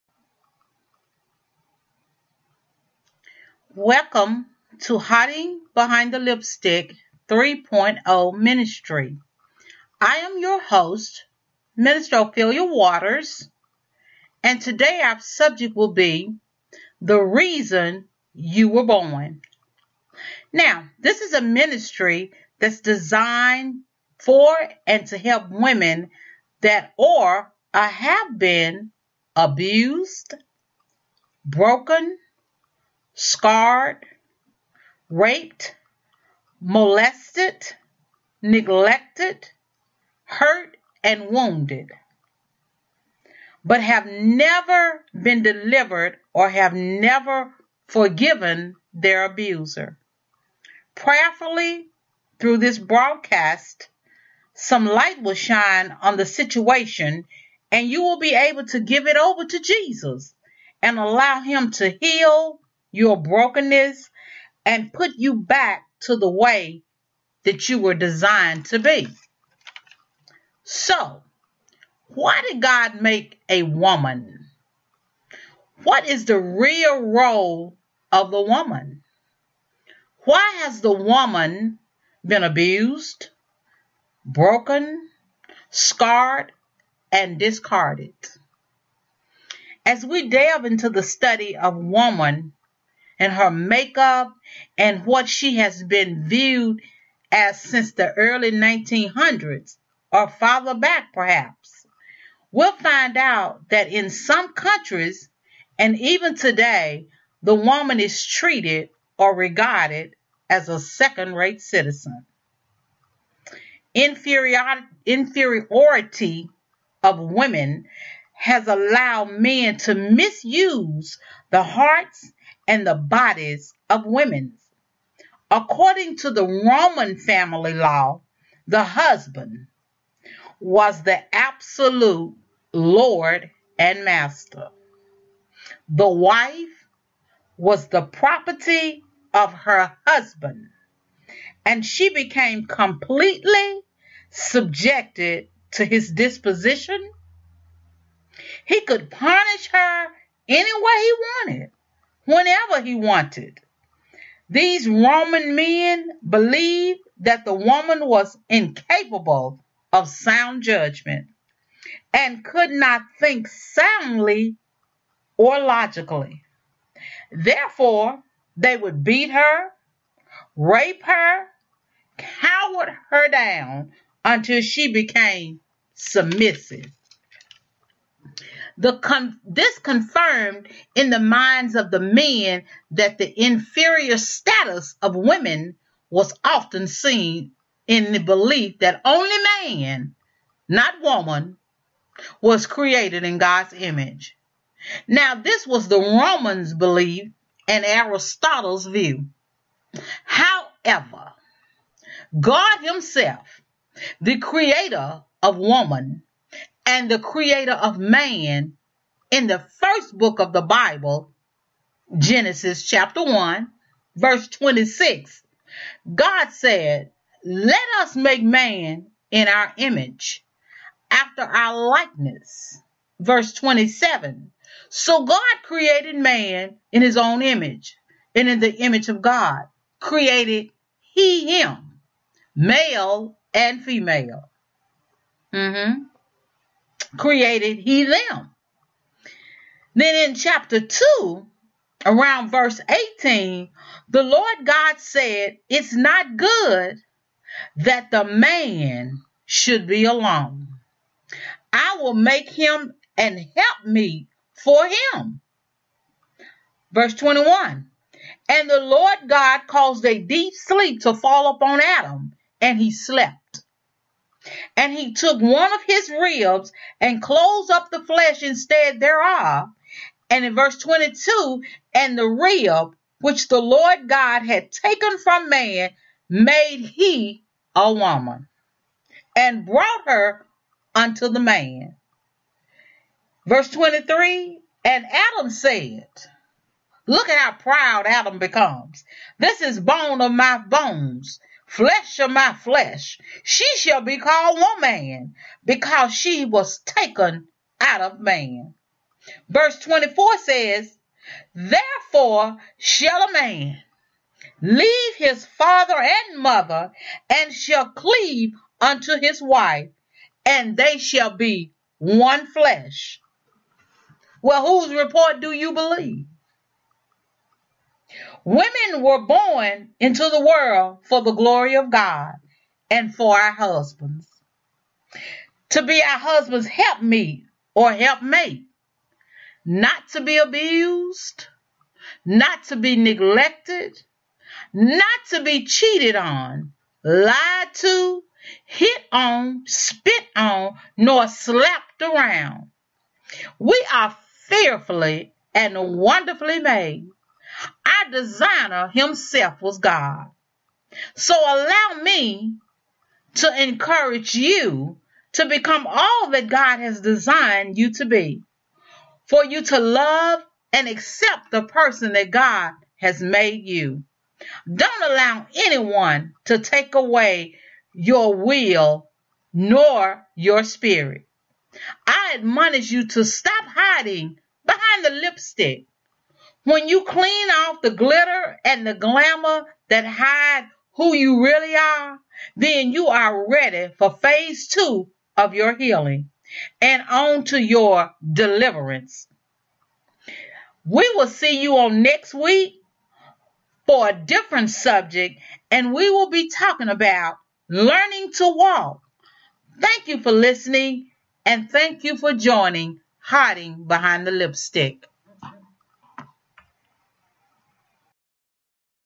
WOMEN THE REASON YOU WERE BORN (made with Spreaker)
Who created the woman and the purpose God created her for. Hiding Behind The Lipstix 3.0 Ministry